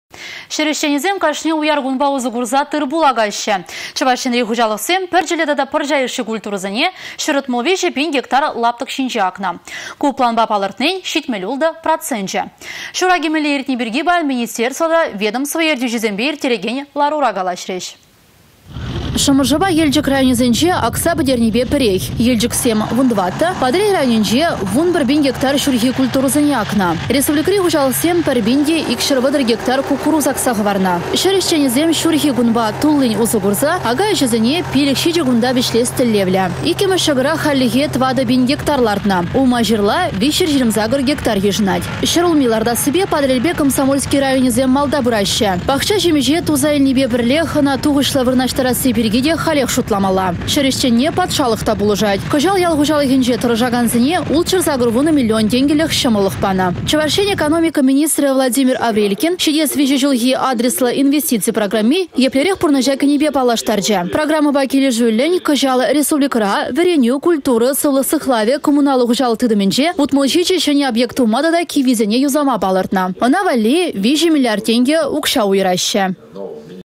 Çур аки пырать
Çĕр ĕçченĕсем кашни уяр кунпа усă курса тыр-пул акаççĕ. Чăваш Енри хуçалăхсем пĕрчĕллĕ тата пăрçа йышши культурăсене 163 пин гектар  лаптăк çинче акнă. Ку планпа палăртнин 76 проценчĕ. Çур аки мĕнле иртни пирки паян министрсемпе тата ведомство ертӳçисемпе ирттерекен ларура калаçрĕç.